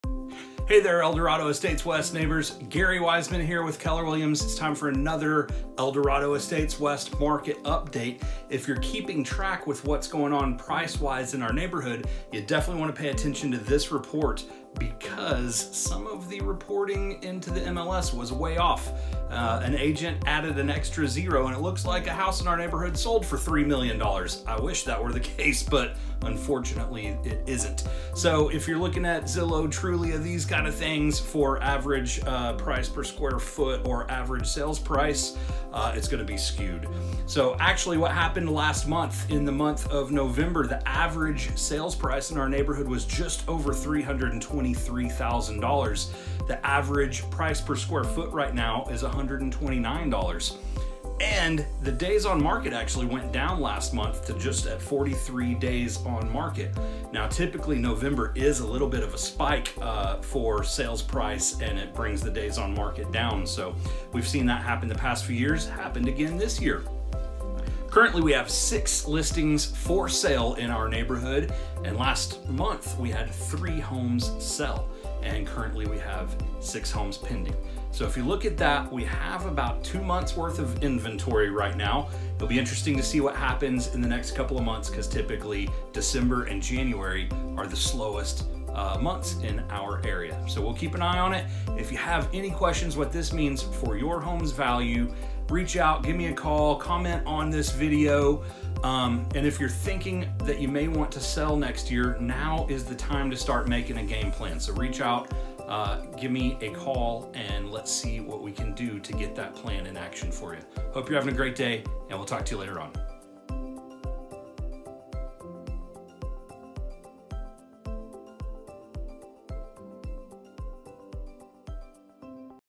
Little Elm Homes for Sale - Eldorado Estates West Market Update November 2019 | Gary Wiseman
This is a real estate update for the Eldorado Estates West neighborhood in Little Elm Tx.
Currently the Average Sales price in Eldorado Estates West is just over $323,000.
The average Price per Sq. Ft. is $129 and the average Days on Market is 43.
There are 6 homes for sale 6 Homes Pending, and 3 homes sold in November, giving us 2 months worth of inventory at the moment.  This means we are currently in a sellers market.